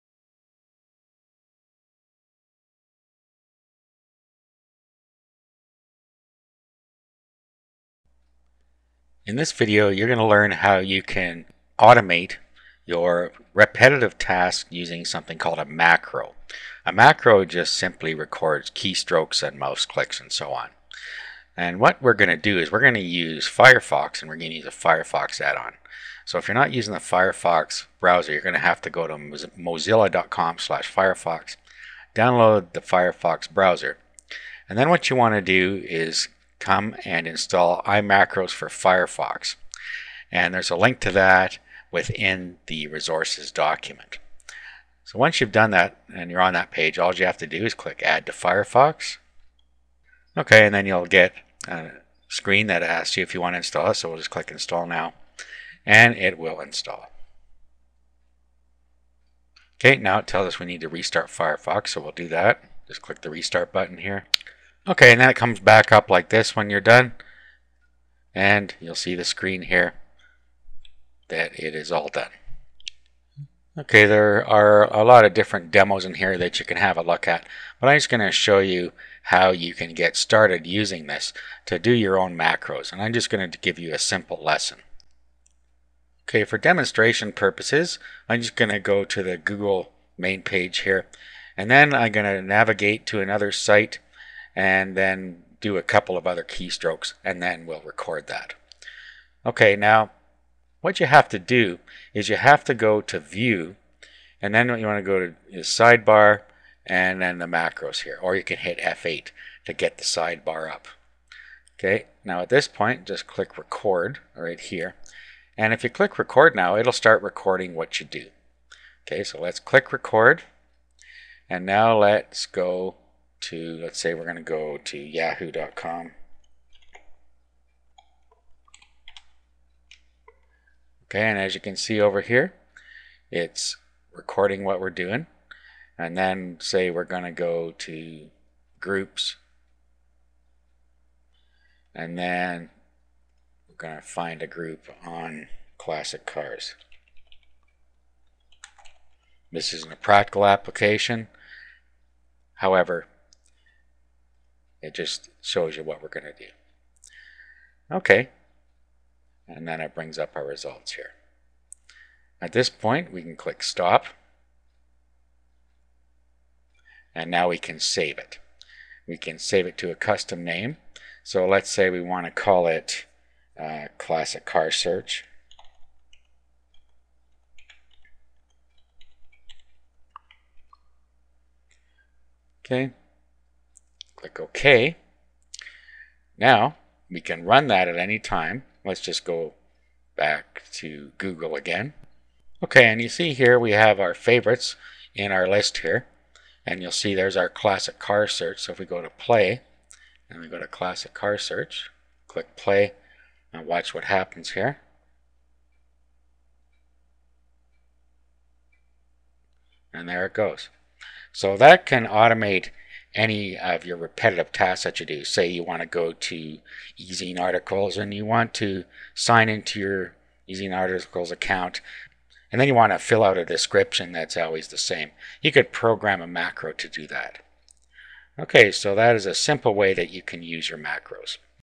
How to Add Macros the the Firefox Web Browser
Learn to use a quick tool in the Firefox® web browser that will enable you to get and use your favorite search terms in an automated format. Thus enabling you to get to those pages much faster.
It is very easy to install, it's free, and it will save you lots of time when searching the internet.